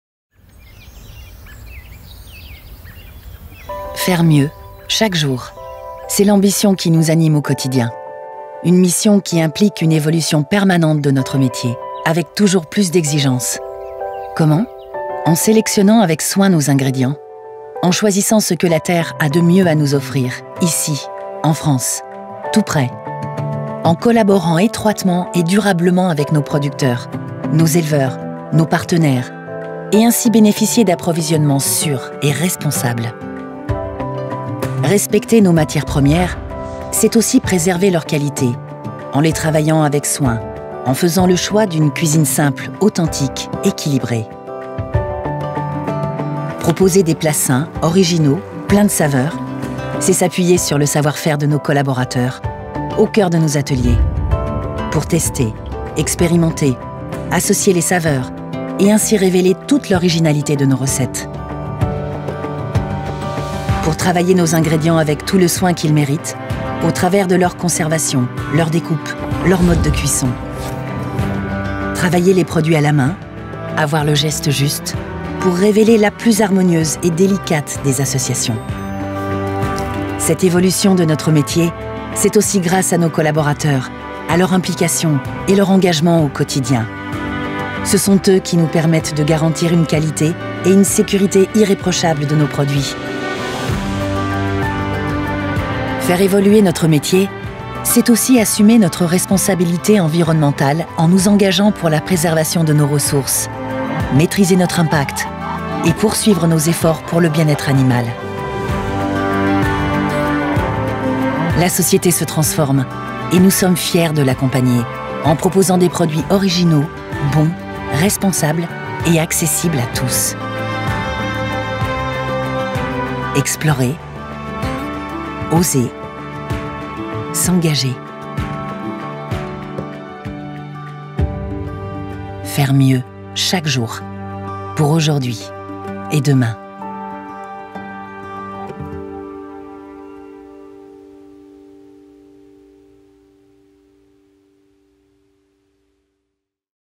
Découvrez Mix Buffet
Découvrez Mix Buffet, nos valeurs et nos engagements au travers de notre film entreprise.

Créé en 1998 par Monsieur Le Hir à Guer en Bretagne, Mix Buffet a choisi le secteur de la salade traiteur pour permettre aux consommateurs de bénéficier d’une alimentation variée et équilibrée, tout en dégustant des produits savoureux. Conscient que manger sainement et avec plaisir est devenu primordial pour les consommateurs, la société met tout en oeuvre pour améliorer ce qu’elle propose.